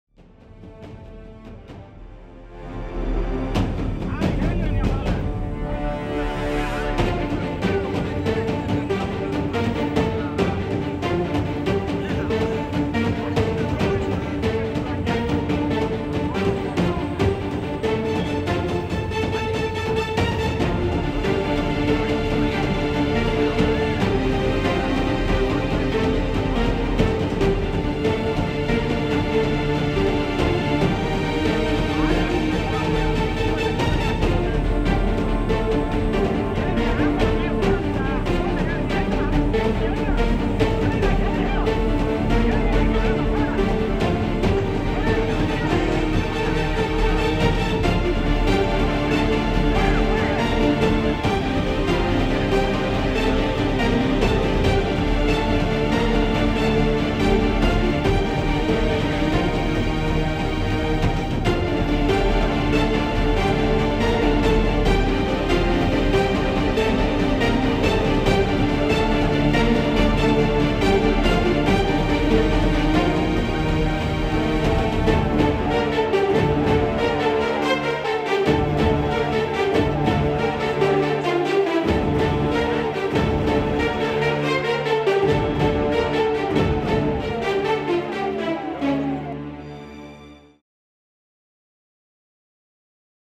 WATCH | Tamil Nadu's great lorry thief chase
The lorry was parked near the rice mill premises as the driver had gone for lunch. Few minutes later, it was being stolen.